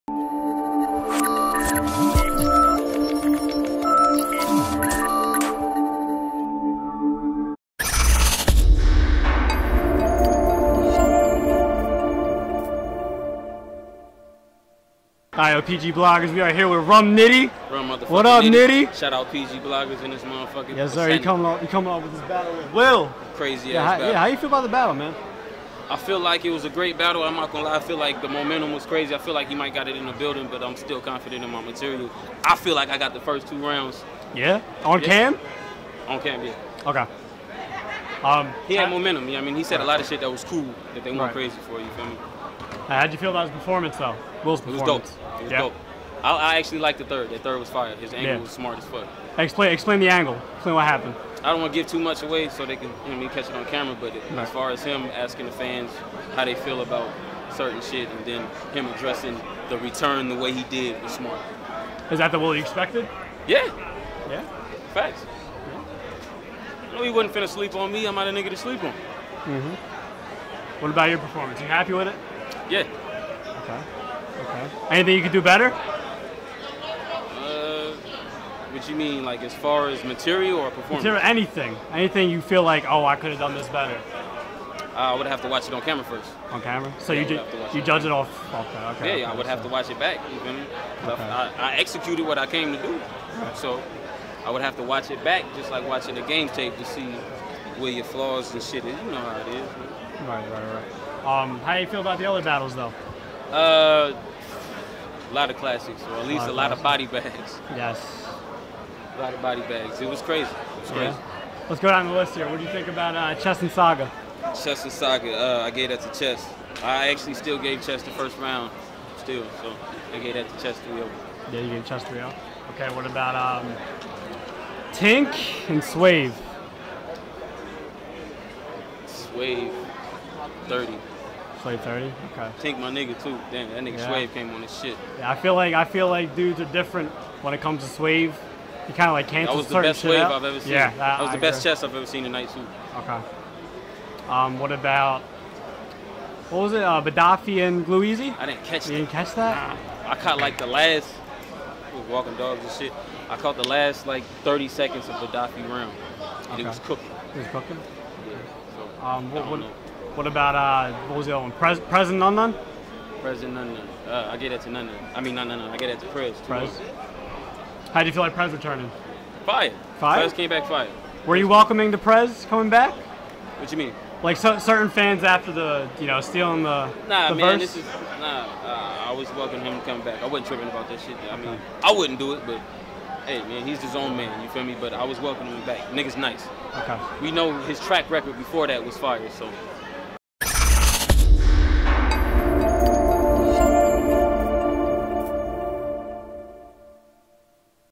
RUM NITTY RECAPS HIS BATTLE WITH ILL WILL
Rum Nitty recaps his battle against Ill Will at URL's Born Legacy 3 Day One.